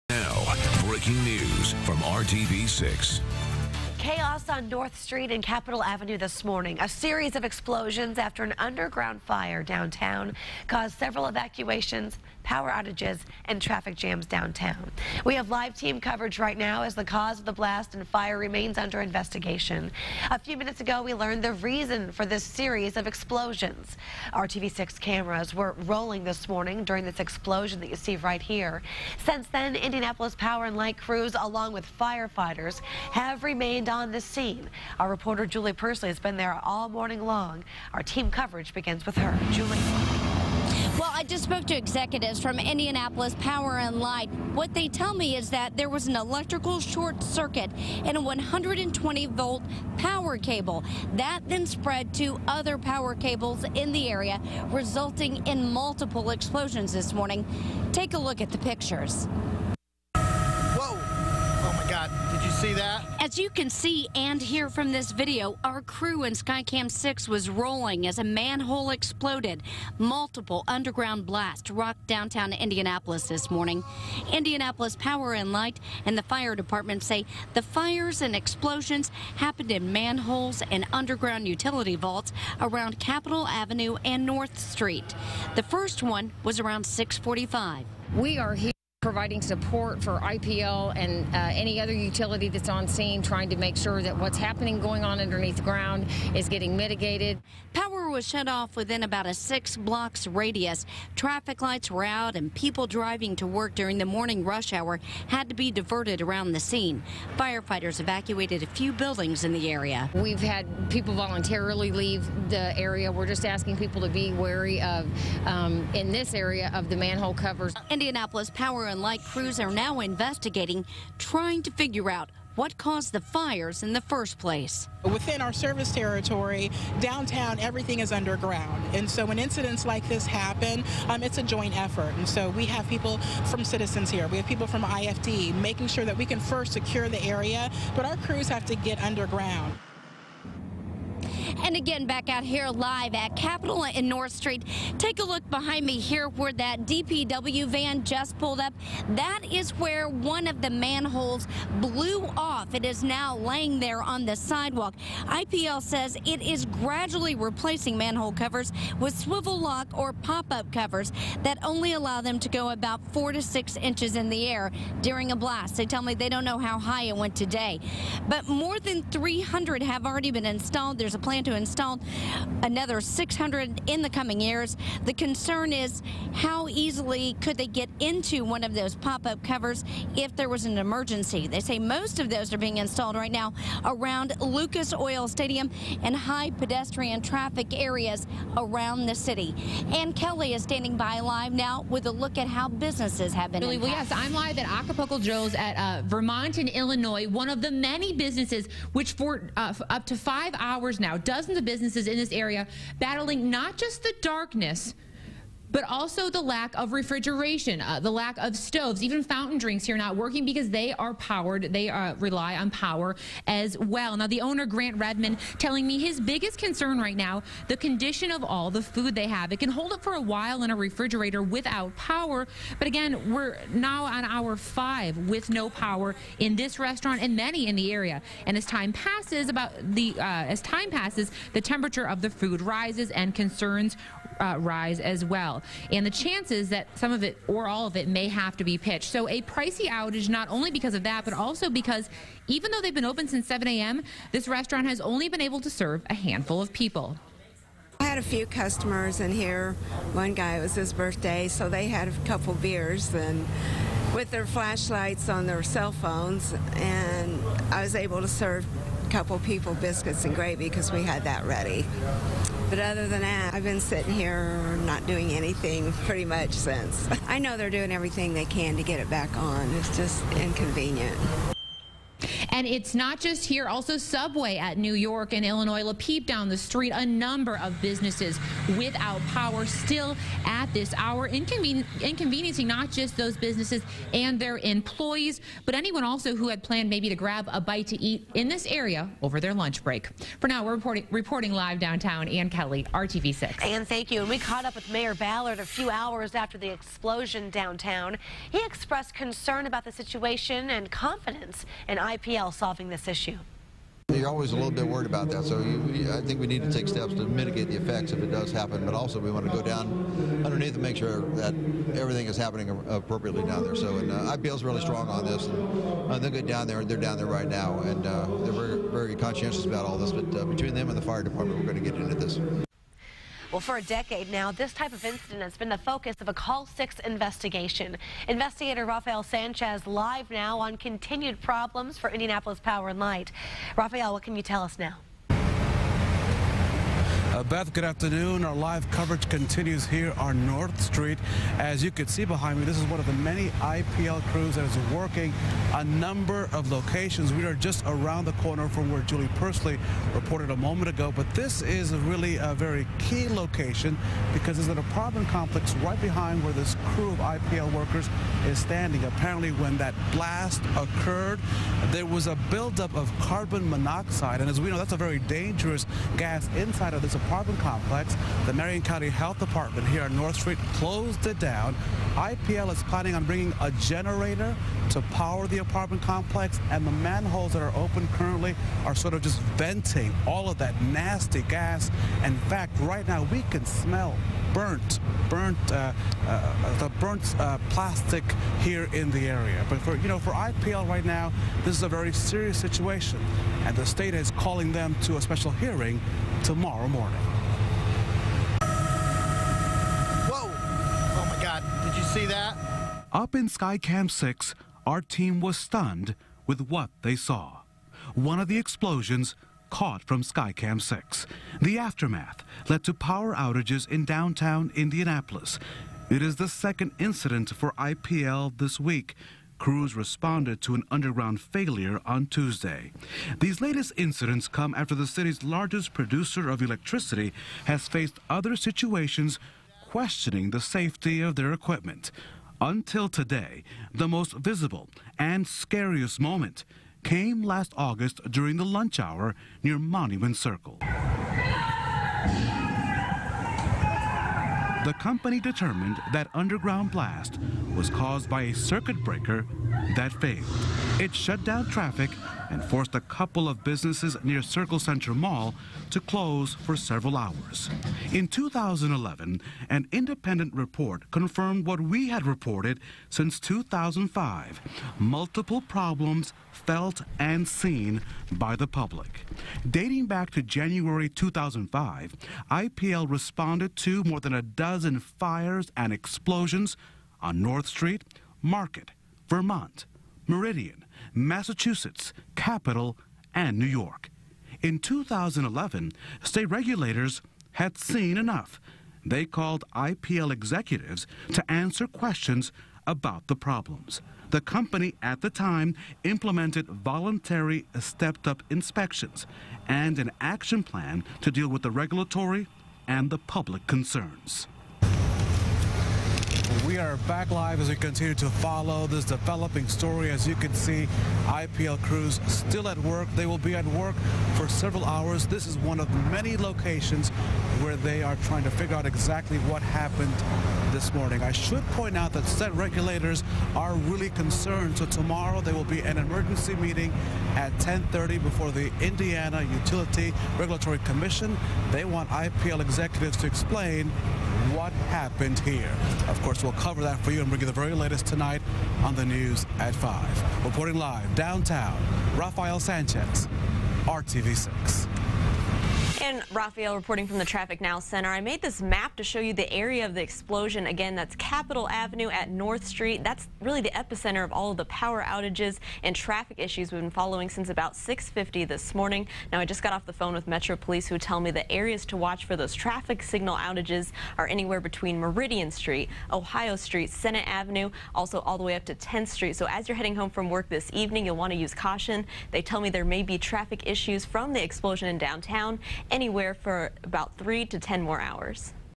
Update: Explosions still impacting life downtown
As of noon, power outages and evacuations were still issues downtown following a series of early-morning explosions.